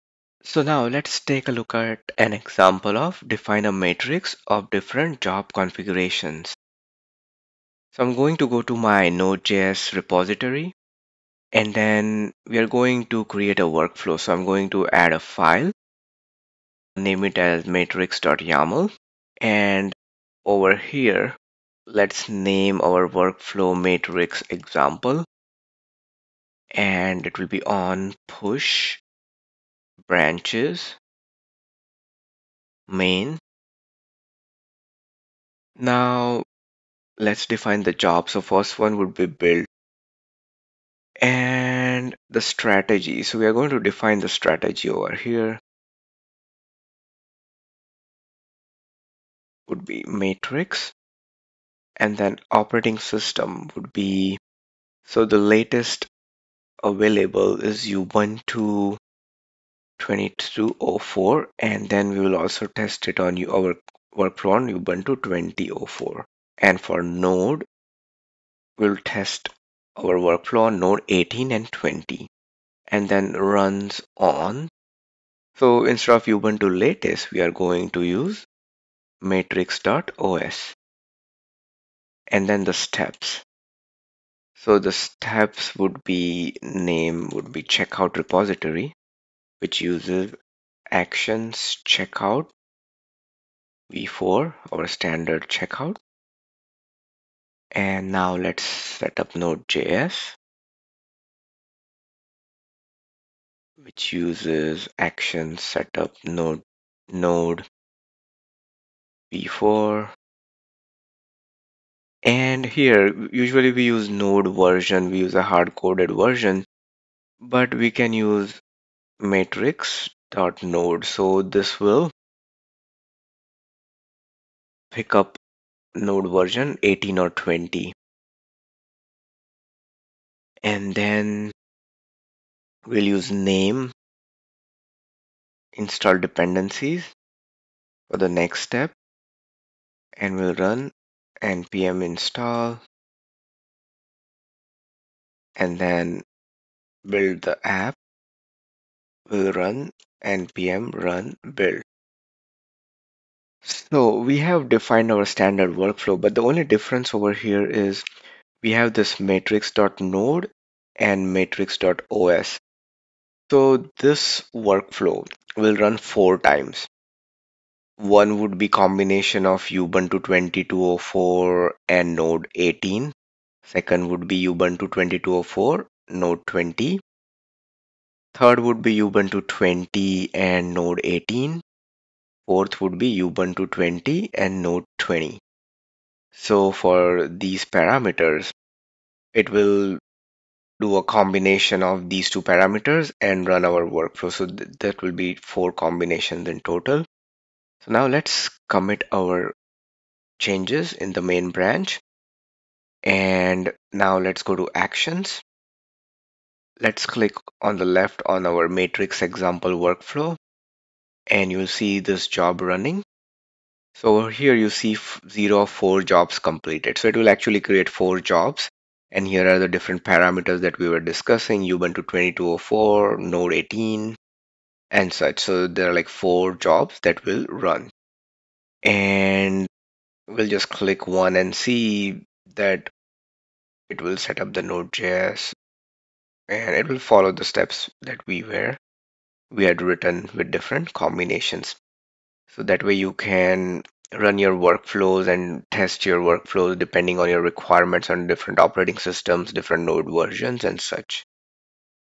Github Actions Certification - Define a matrix of different job configurations
Domain 1: Author and maintain workflows
Section: Manage workflow runs
Define a matrix of different job configurations